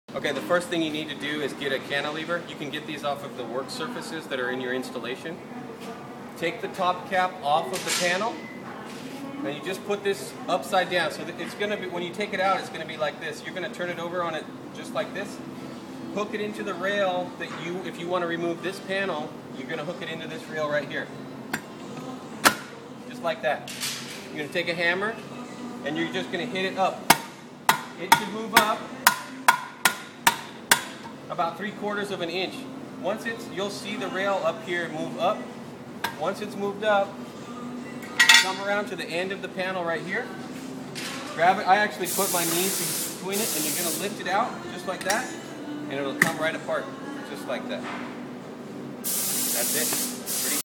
How to remove a Haworth Panel
Haworth Unigroup panel systems use rails and hinges.  You generally can find a cantilever in the install and use it as a tool to remove the rails from the panels.  Make sure to remove the power connectors and top caps prior to panel removal.